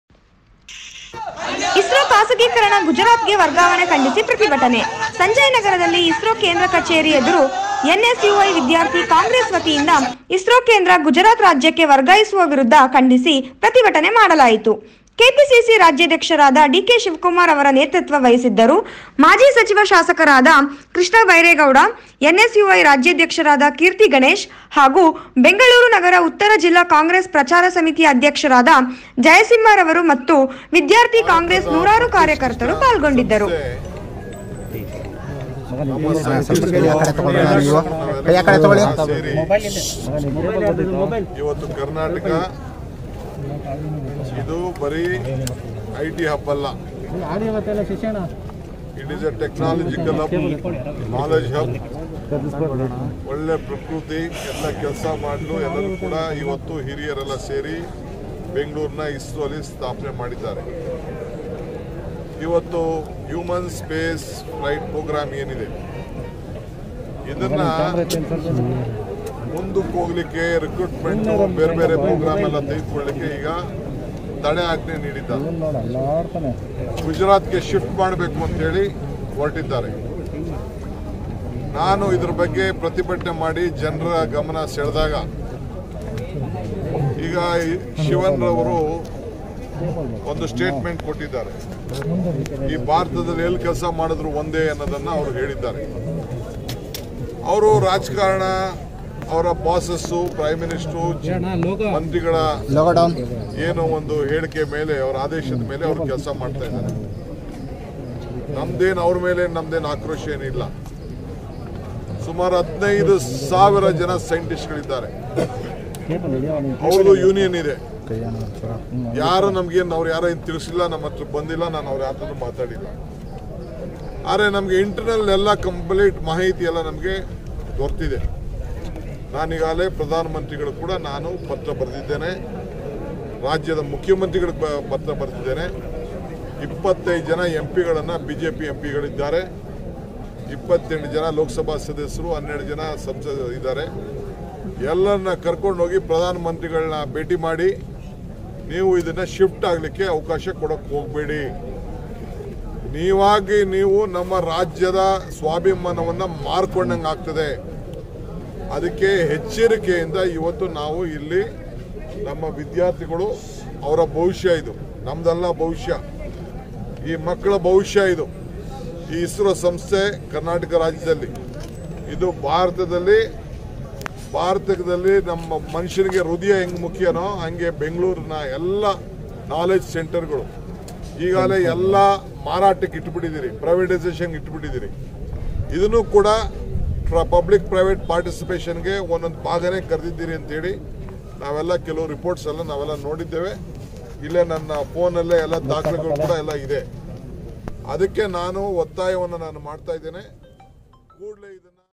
ಎನ್.ಎಸ್.ಯು.ಐ.ವಿದ್ಯಾರ್ಥಿ ಕಾಂಗ್ರೆಸ್ ವತಿಯಿಂದ ಇಸ್ರೋ ಕೇಂದ್ರ ಗುಜರಾತ್ ರಾಜ್ಯಕ್ಕೆ ವರ್ಗಾಯಿಸುವ ವಿರುದ್ದ ಖಂಡಿಸಿ
ಎನ್.ಎಸ್.ಯು.ಐ.ವಿದ್ಯಾರ್ಥಿ ಕಾಂಗ್ರೆಸ್ ವತಿಯಿಂದ ಇಸ್ರೋ ಕೇಂದ್ರ ಗುಜರಾತ್ ರಾಜ್ಯಕ್ಕೆ ವರ್ಗಾಯಿಸುವ ವಿರುದ್ದ ಖಂಡಿಸಿ ಪ್ರತಿಭಟನೆ ಮಾಡಲಾಯಿತು.
ಕೆ.ಪಿ.ಸಿ.ಸಿ ರಾಜ್ಯಾಧ್ಯಕ್ಷರಾದ ಡಿ.ಕೆ.ಶಿವಕುಮಾರ್ ರವರ ಎನ್.ಎಸ್.ಯು.ಐ.ರಾಜ್ಯಾಧ್ಯಕ್ಷರಾದ ಕೀರ್ತಿ ಗಣೇಶ್ ಹಾಗೂ ವಿದ್ಯಾರ್ಥಿ ಕಾಂಗ್ರೆಸ್ ನೂರಾರು ಕಾರ್ಯಕರ್ತರು ಪಾಲ್ಗೊಂಡಿದ್ದರು